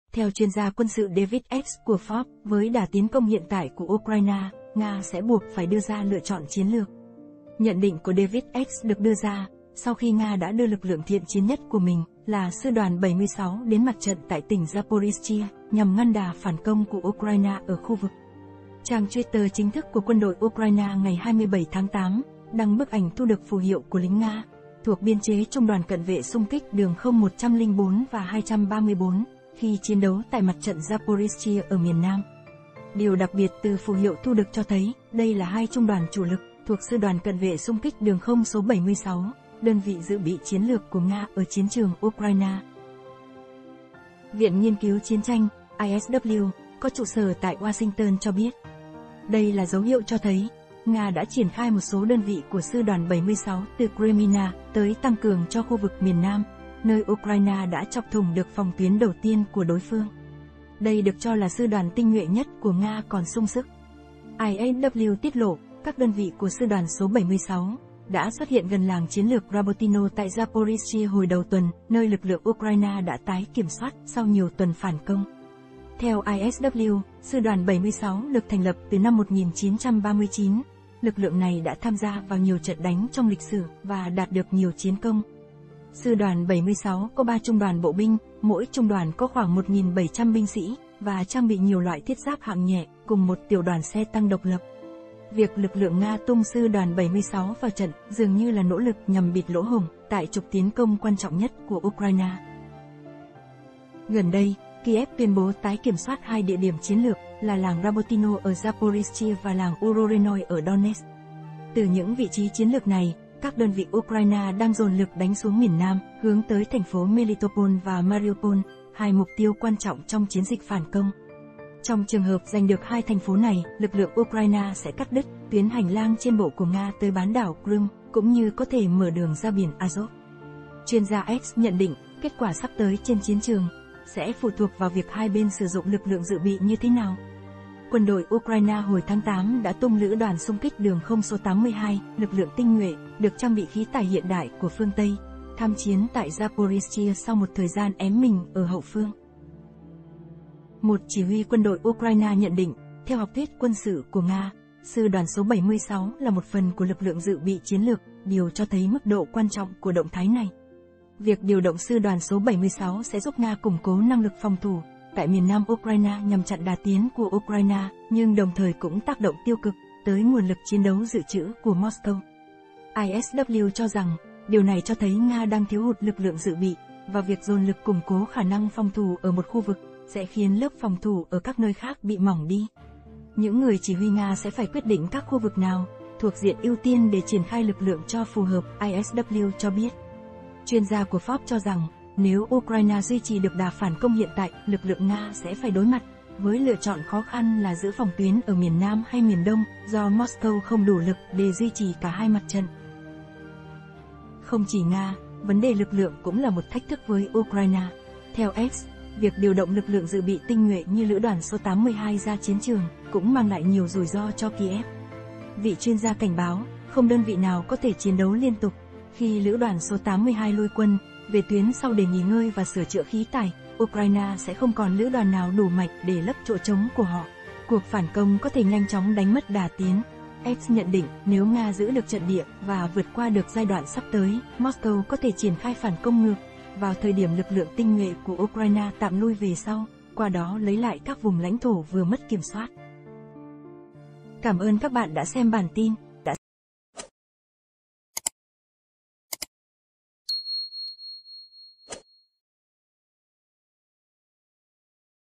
MOSCOW VÀ KIEV ĐỀU ĐÃ TUNG LỰC LƯỢNG TINH NHUỆ, CUỘC PHẢN CÔNG NÀY SẼ NHANH CHÓNG CÓ KẾT QUẢ?
Nhận định của David Axe được đưa ra, sau khi Nga đã đưa lực lượng thiện chiến nhất của mình, là Sư đoàn 76 đến mặt trận tại tỉnh Zaporizhzhia, nhằm ngăn đà phản công của Ukraine ở khu vực.